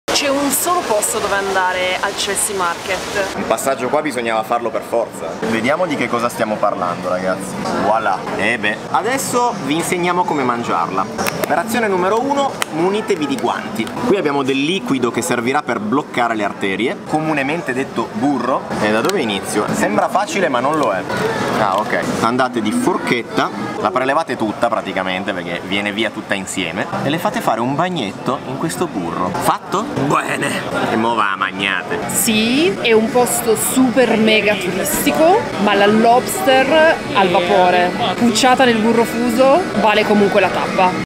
ASTICE annegato nel BURRO a New York 🦞🧈
Lobster Place al Chelsea Market a New York è il posto dove mangiare l’astice cotto al vapore, ovviamente dopo averlo annegato nel burro fuso